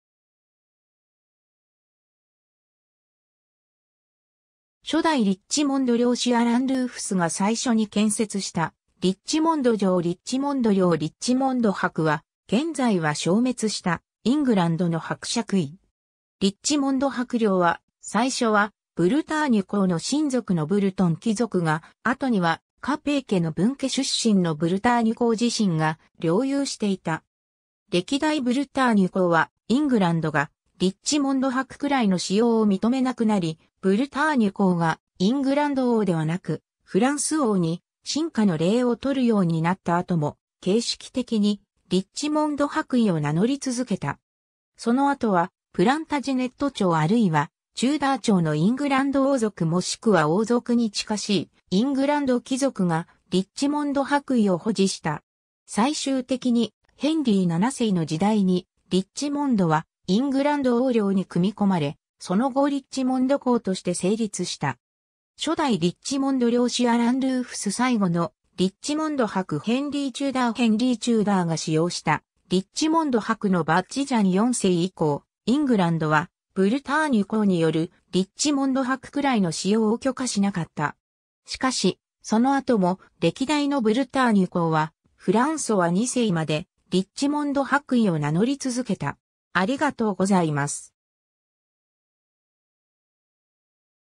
リッチモンド伯
初代リッチモンド領主アラン・ルーフスが最初に建設したリッチモンド城 リッチモンド領 リッチモンド伯（Earl of Richmond）は、現在は消滅したイングランドの伯爵位。
リッチモンド伯領は、最初はブルターニュ公の親族のブルトン貴族が、後にはカペー家の分家出身のブルターニュ公自身が領有していた。
歴代ブルターニュ公は、イングランドがリッチモンド伯位の使用を認めなくなり、ブルターニュ公がイングランド王ではなく、フランス王に臣下の礼を取るようになった後も、形式的にリッチモンド伯位を名乗り続けた。
その後はプランタジネット朝あるいはテューダー朝のイングランド王族もしくは王族に近しいイングランド貴族がリッチモンド伯位を保持した。
最終的に、ヘンリー7世の時代に、リッチモンドはイングランド王領に組み込まれ、その後リッチモンド公として成立した。
 初代リッチモンド領主アラン・ルーフス 最後のリッチモンド伯ヘンリー・チューダー（後のイングランド王ヘンリー7世） ヘンリー・チューダーが使用したリッチモンド伯のバッジ ジャン4世以降、イングランドはブルターニュ公によるリッチモンド伯位の使用を許可しなかった。
しかし、その後も歴代のブルターニュ公はフランソワ2世までリッチモンド伯（リッシュモン伯）位を名乗り続けた。